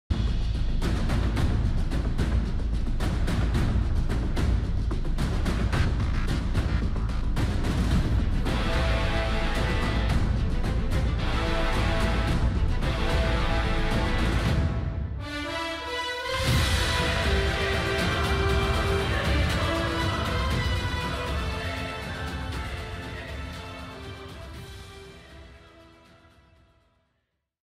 Leap of Faith
So i started playing priest, and i don't know, i am new to this, but this gave me great vibes